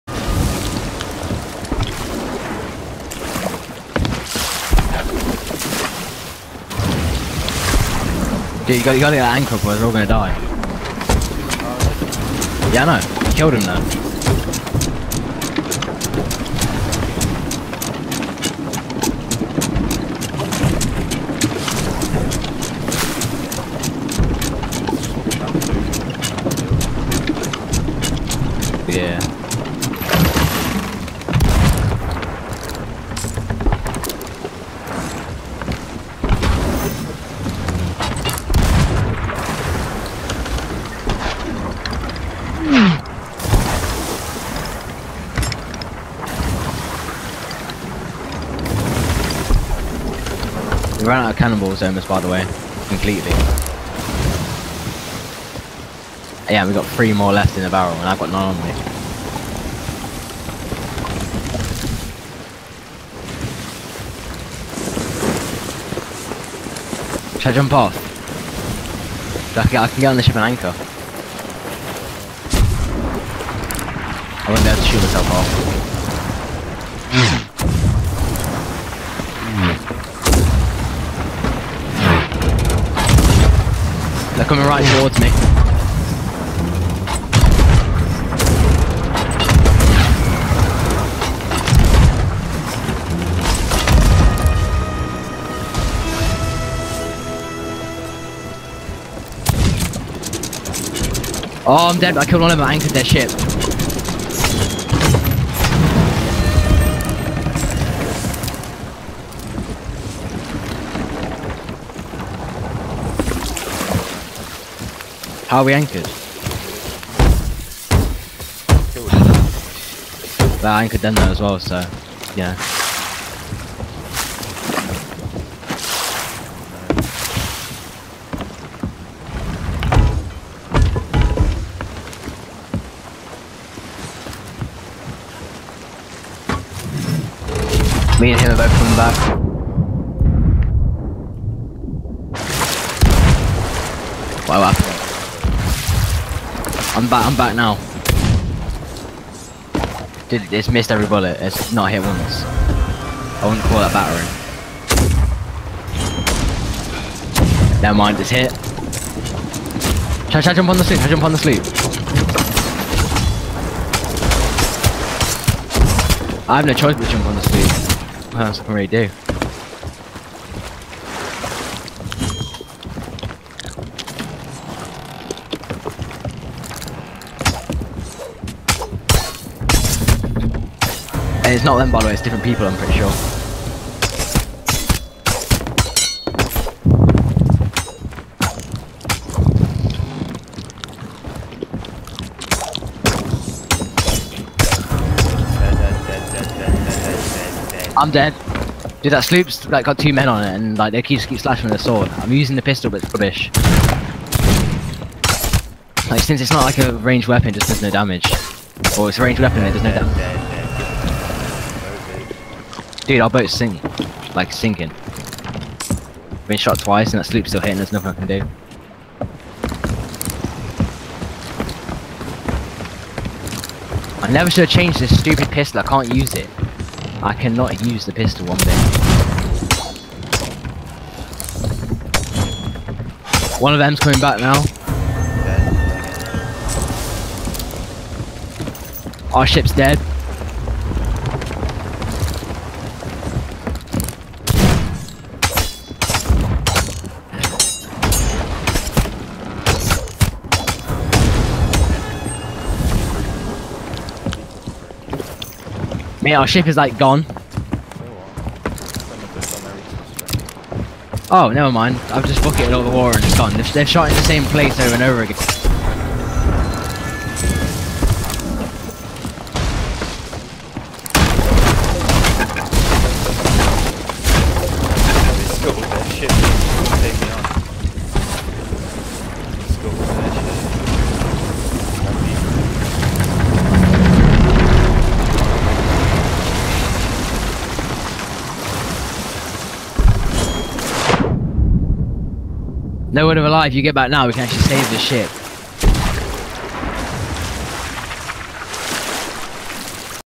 Capt Molehill takes on some Pirate legends?
some crap pirate legends who cant fight end up having to scuttle there ship because they cant kill Capt Molehill